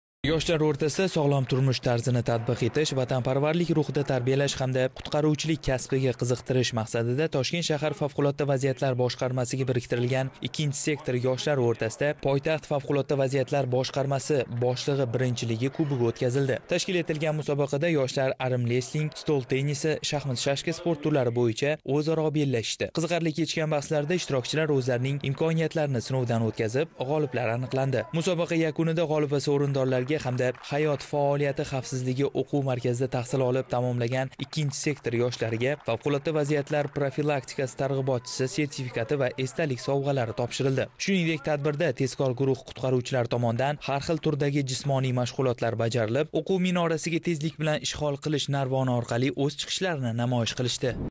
Toshkent shahar FVBga biriktirilgan yoshlar o'rtasida sport musobaqalari o‘tkazildi
Toshkent shahar Favqulodda vaziyatlar boshqarmasiga biriktirilgan 2-sektor yoshlari o'rtasida boshqarma boshlig'i birinchiligi bo'yicha o‘tkazilgan sport musobaqalari tafsiloti.